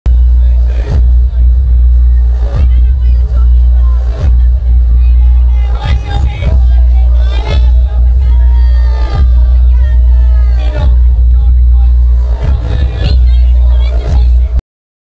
Propaganda - Who is the best artist or band in 2012 and Why?
O2 asked fans at O2 Academy Birmingham: "Who is the best artist or band in 2012 and Why?"